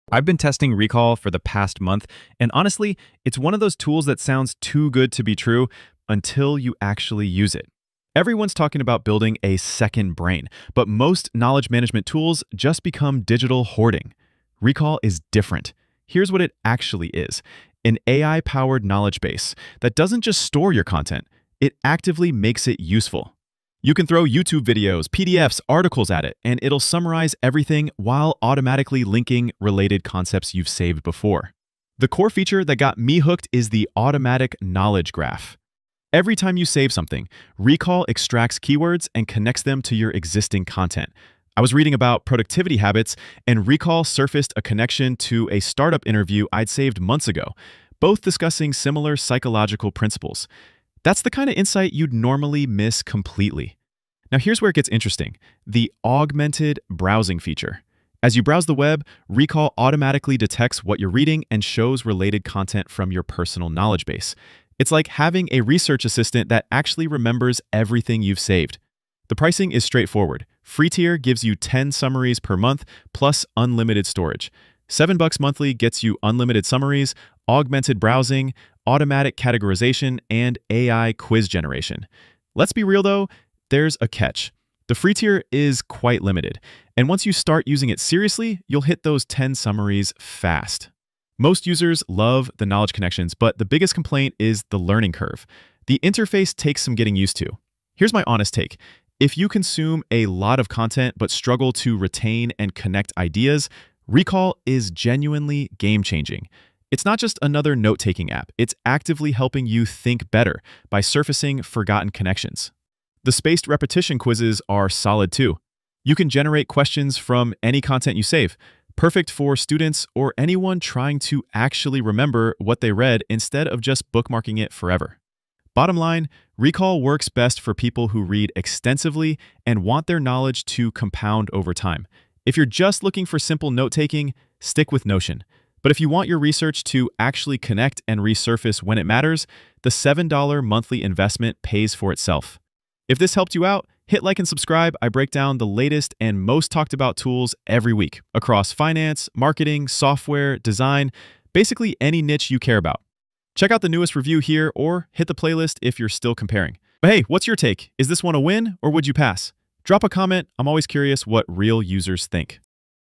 Recall AI Review: Build a Second Brain with Augmented Browsing (2026)
🎬 In This Video: Hook
 Tired of endless tabs and zero memory? See how Recall turns content into insights you actually remember.
Video Summary
 In this in‑depth review, we unpack Recall AI—the tool that goes far beyond note‑taking. Learn how its AI‑powered summaries work across YouTube, articles, PDFs, and more to build a dynamic knowledge base. We’ll explore the standout features:
Automatic content summaries and knowledge graph linking for powerful idea connections (toolfinder.net, altagic.com, aipure.ai, geeky-gadgets.com, opentools.ai, geeky-gadgets.com, futurepedia.io)

Augmented browsing that surfaces related lessons as you browse online (toolfinder.co)

Spaced repetition quizzes to turn saved content into long‑term memory (aihuntlist.com)

We’ll also dive into the pricing tiers—free, $7–10/month unlimited summaries, and lifetime/business plans (aihungry.com)—along with real user pros and cons like the learning curve and value versus simpler tools like Notion.
Whether you're a student, researcher, or lifelong learner, this video shows why Recall is one of the best AI tools of 2026.
Call‑to‑Action
Curious? Try Recall through the links below, then share your take in the comments.
🛠️ Tools Mentioned
getrecall.ai (Recall) ‎